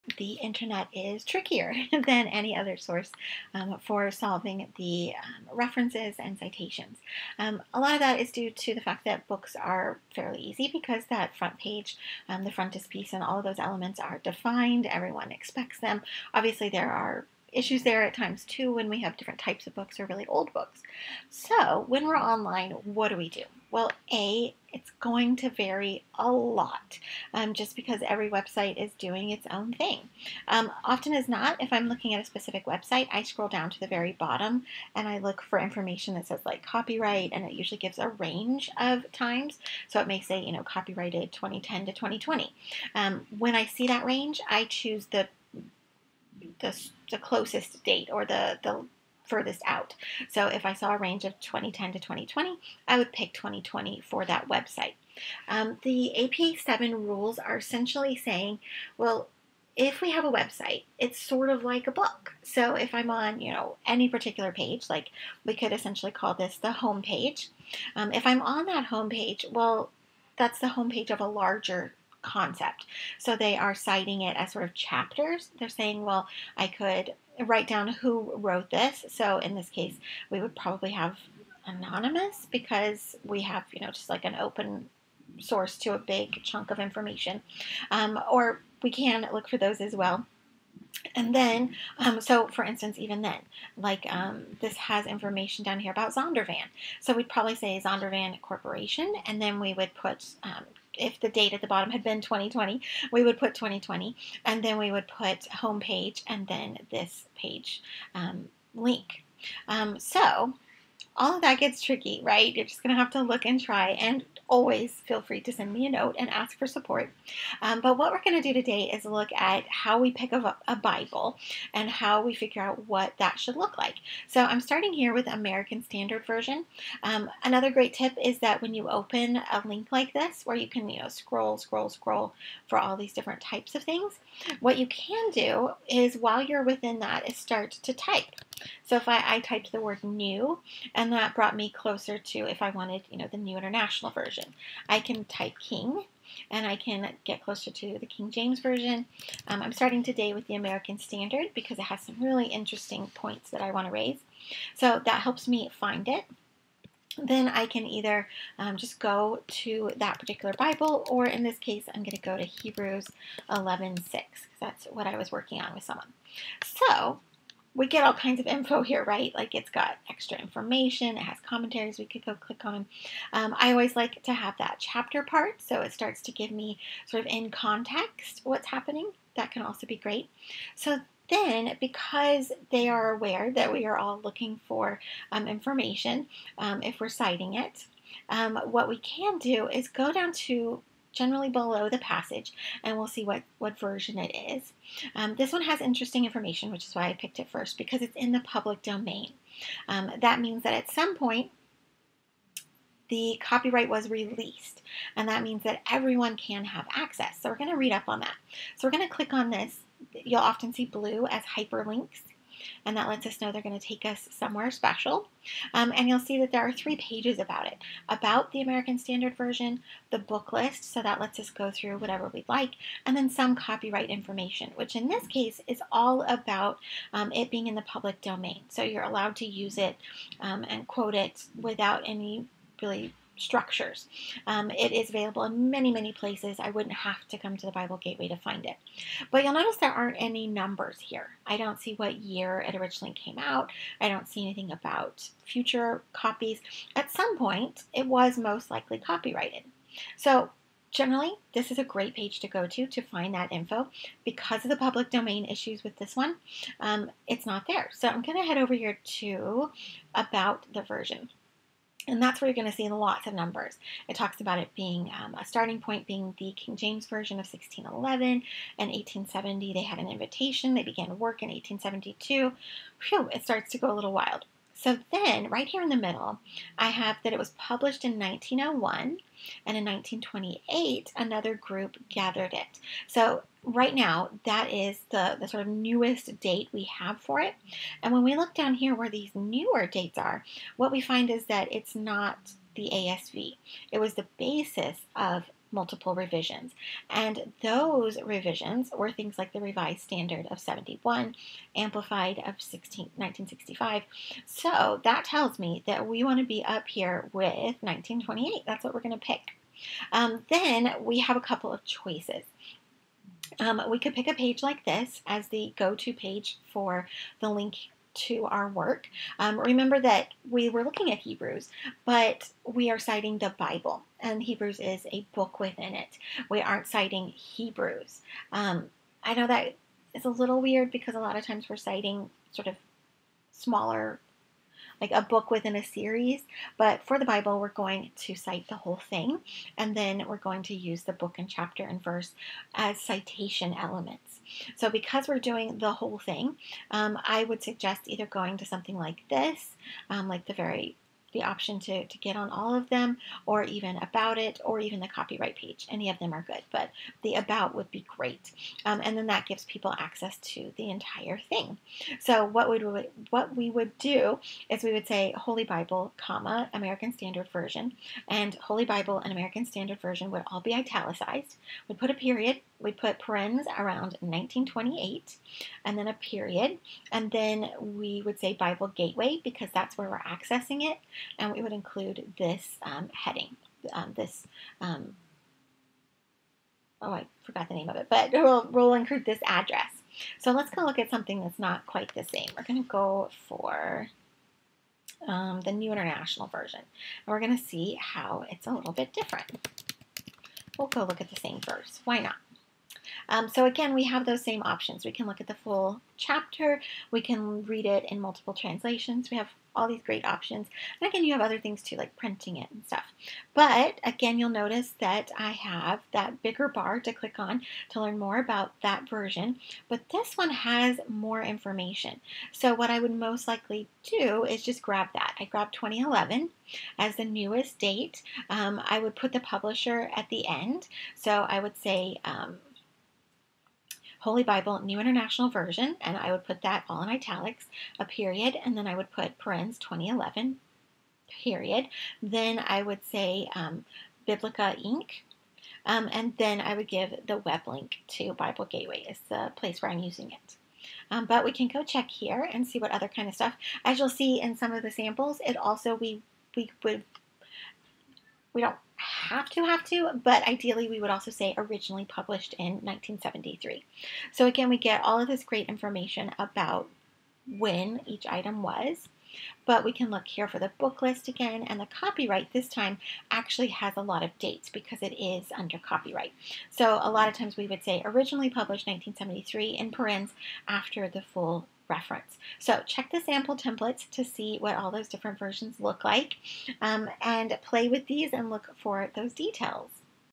Bible References Online
There are lots of great supports on websites like Bible Gateway, but it can still be a bit tricky to find all we need about a resource. This video looks at the American Standard Version and the New International Version to help you find these details for references and citations.